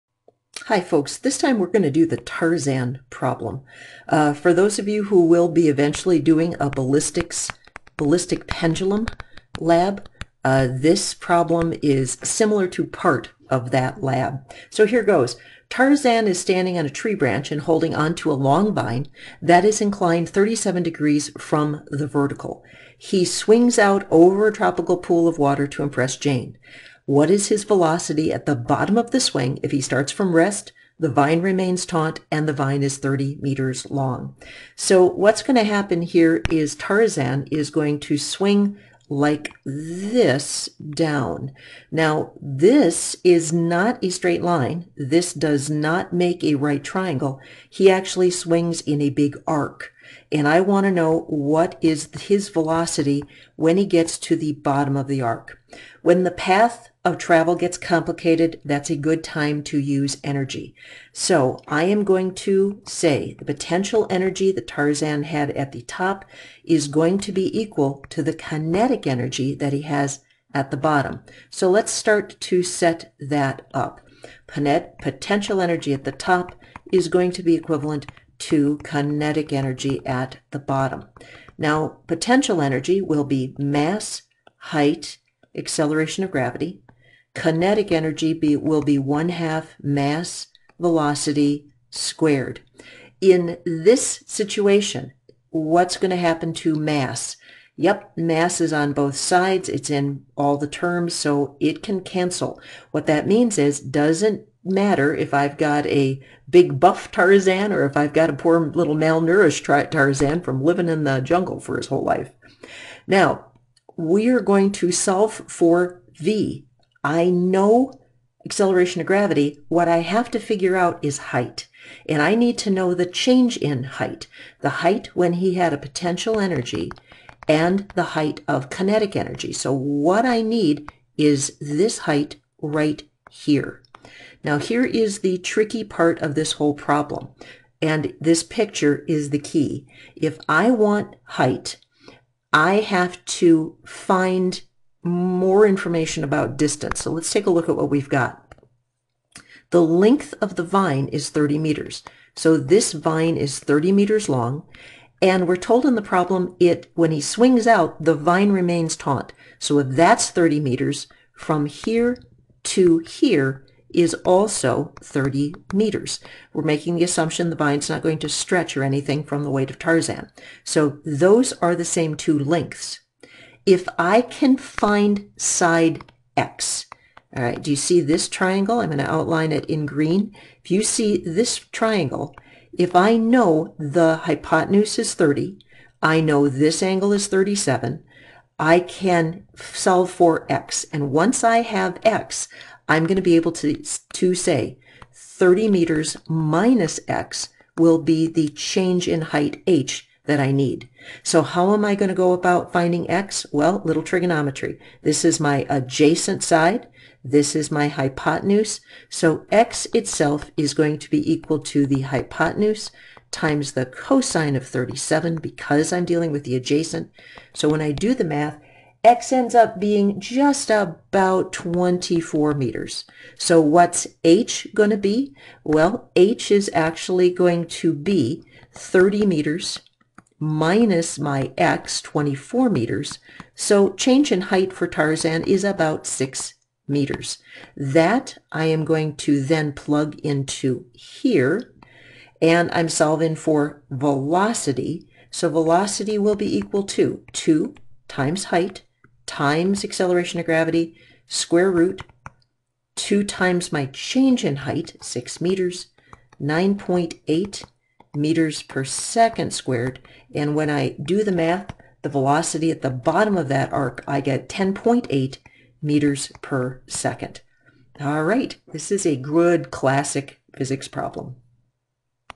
PP 7-8 Tarzan Problem
In this classic physics problem Tarzan's energy gets transformed from potential energy to kinetic energy.  If you are working with a ballistic pendulum, this problem is the same as one portion of those calculations.